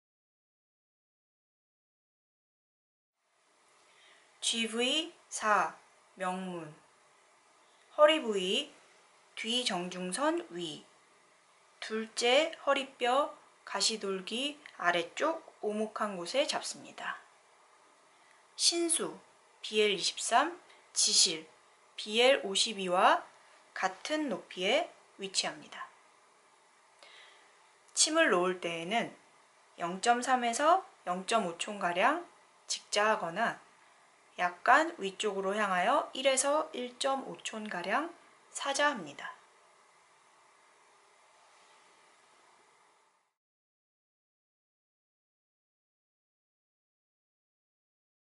[KMCRIC 표준경혈 DB] GV4 명문(命門)
[부위]
허리부위, 뒤정중선(posterior median line) 위, 넷째 허리뼈 가시돌기(spinous process of the 4th lumbar vertebra) 아래쪽 오목한 곳.

In the lumbar region, in the depression inferior to the spinous process of the 4th lumbar vertebra (L4), on the posterior median line.

[취혈]
먼저 양쪽 엉덩뼈능선(iliac crest)의 가장 높은 지점을 연결하는 선의 중점에서 넷째 허리뼈 가시돌기를 찾고, 그 아래쪽 오목한 곳에서 취혈한다.

Note: GV3 can be located by first palpating the highest points of the ilica crests and the spinous process of the 4th lumbar vertebra (T4) can then be found at the midpoint of the highest points of both iliac crests.

[침구법]
- 直刺 0.3～0.5寸
- 斜刺 0.5～1寸 (약간 上向)

★ 한의학 뉴스, 학회, 연구 동향 등 더 풍부하고 다양한 한의학 정보를 한의약융합연구정보센터 홈페이지에서 만나보세요.